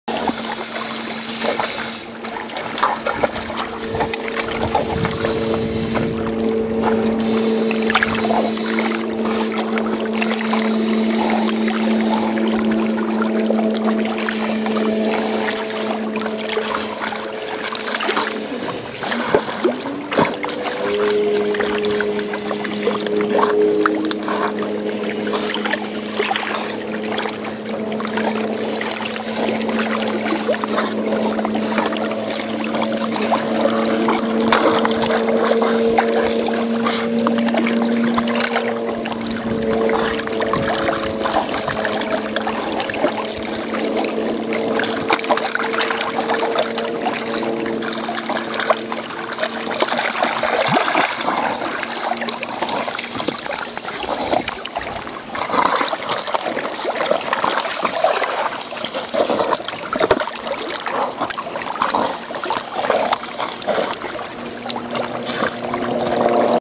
Violet swimming at Cherry Lake
Miss Violet taking a swim at Cherry Lake, CA June 15, 2013.  She found herself a stick to play with of course.